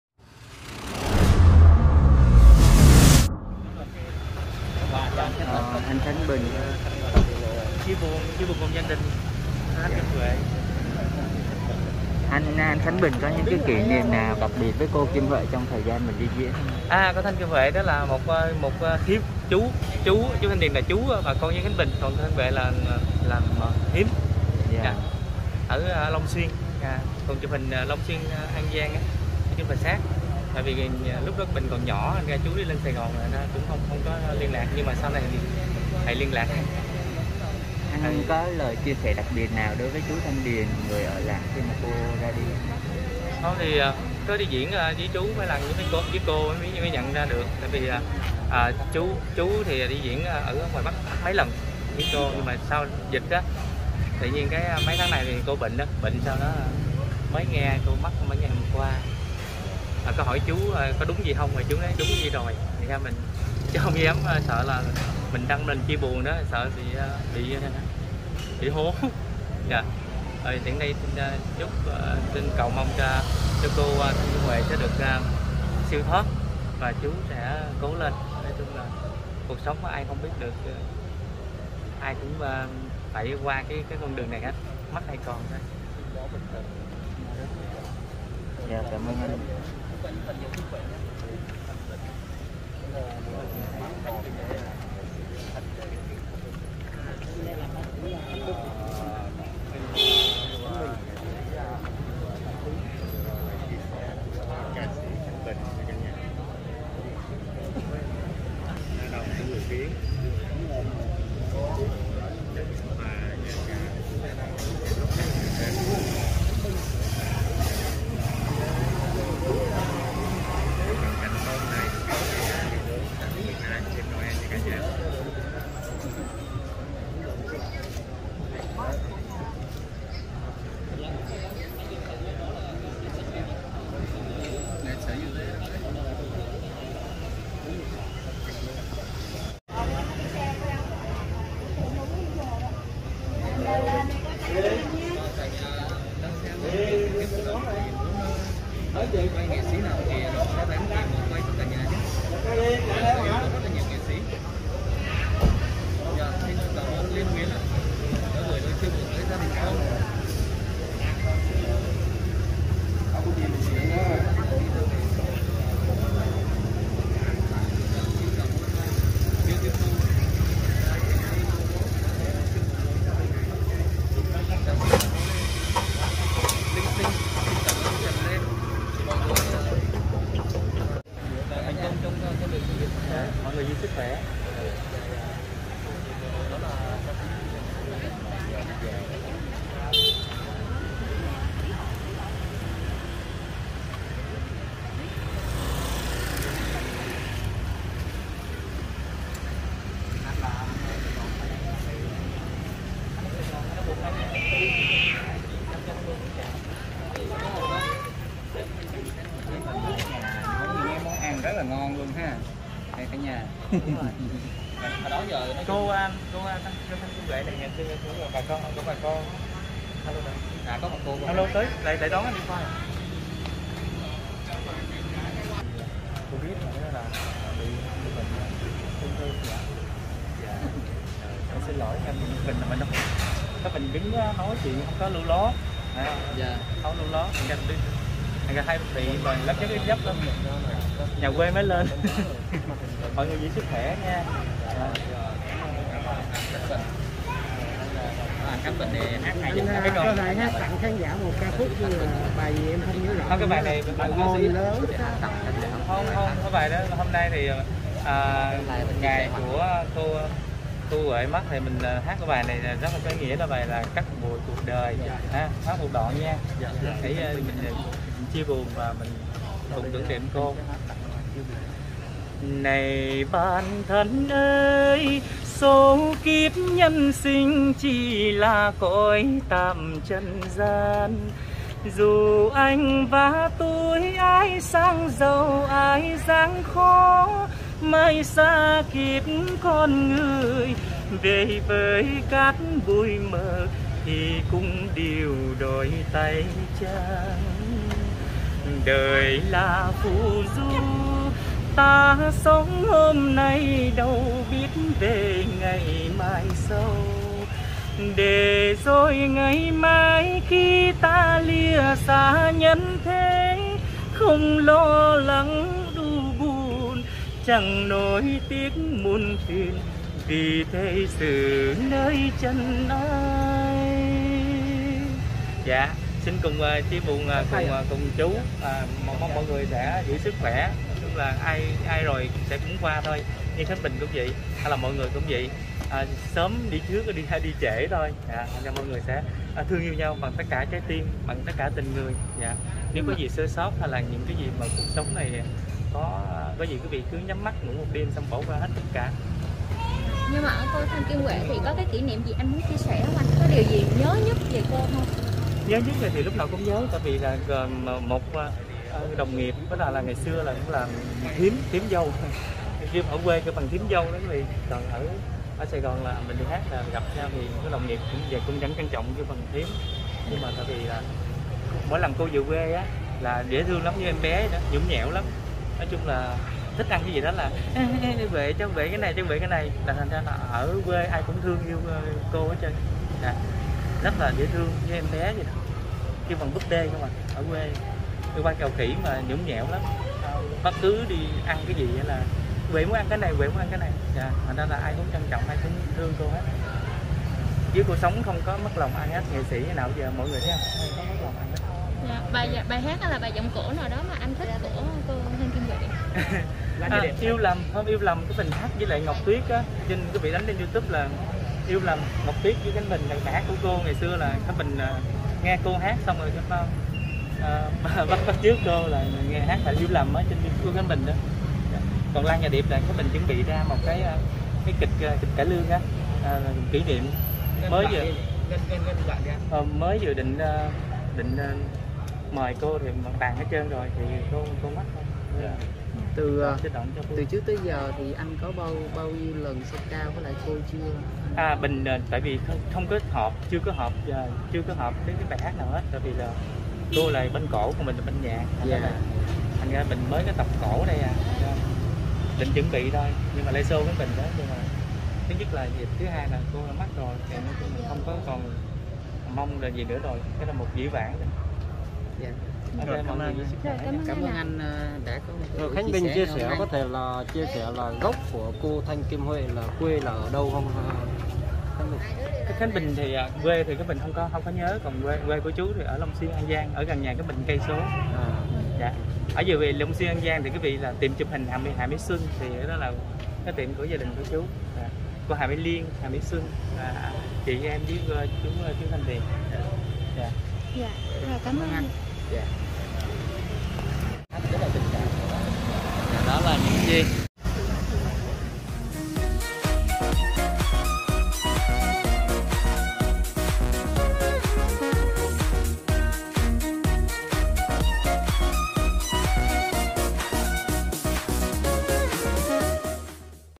Khánh Bình đến viếng và hát tưởng nhớ Thanh Kim Huệ, tiết lộ cô Huệ khi ở nhà rất nhõng nhẽo
Khánh Bình hát tưởng nhớ Thanh Kim Huệ, tiết lộ cô Huệ khi ở nhà rất nhõng nhẽo, như búp bê